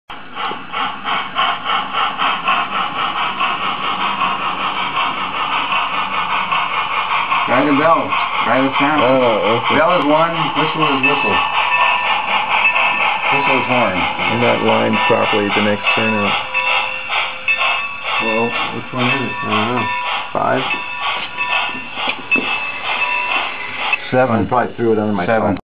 Switcher on Andy's Yard
Messing with a nice Lifelike Steam Switcher on Andy's Layout